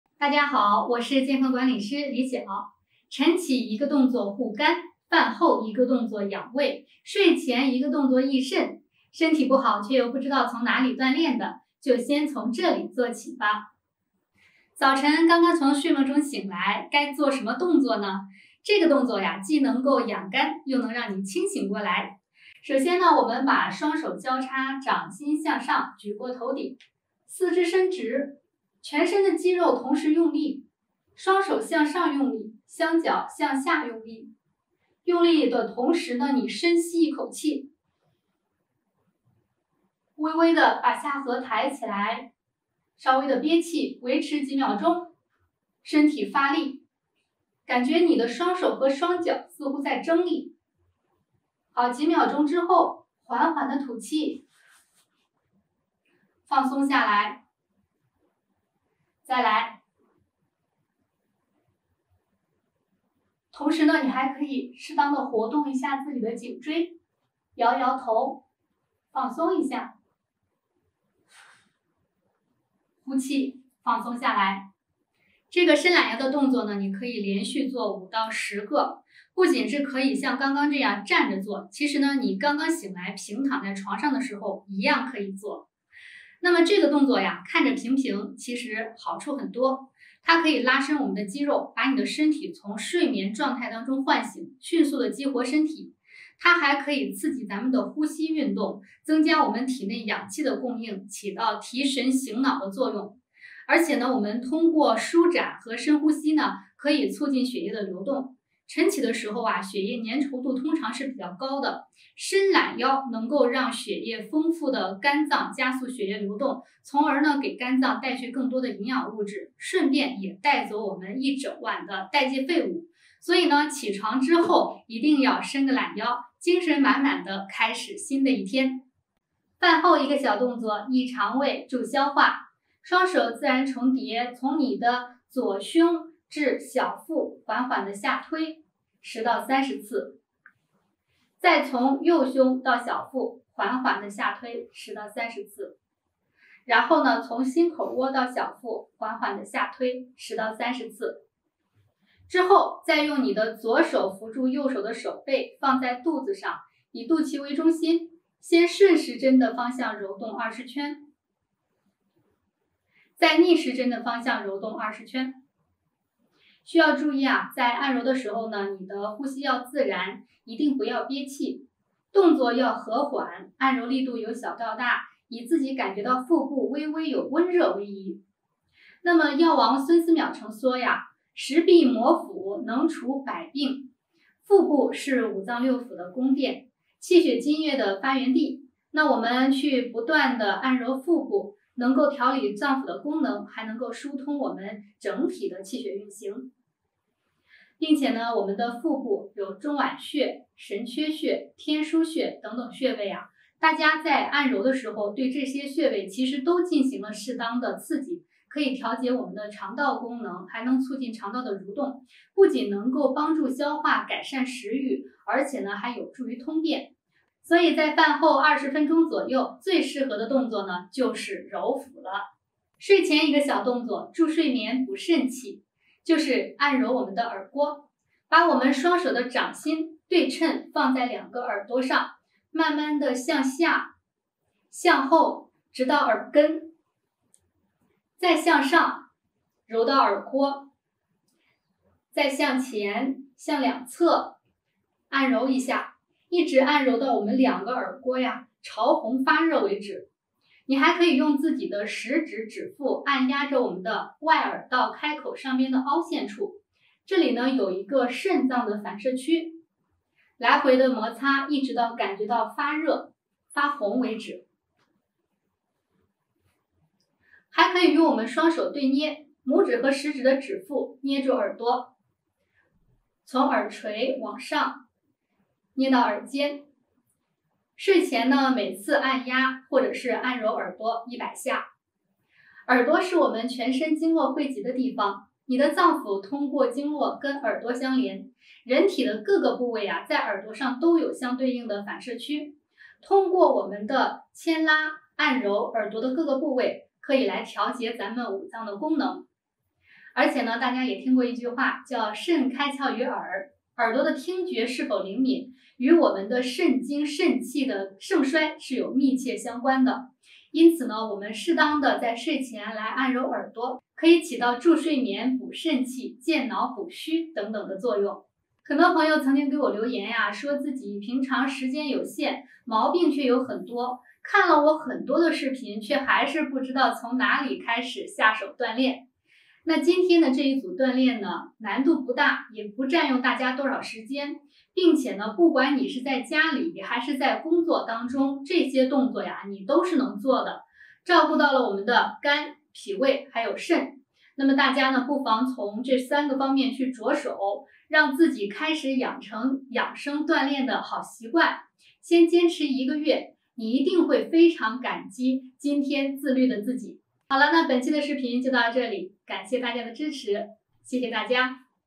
晨起1個動作護肝，飯後1個動作養胃，睡前1個動作益腎！ 【健康管理師李曉】
清晨一個動作護肝，飯後一個動作養胃，走路一個動作益腎，別錯過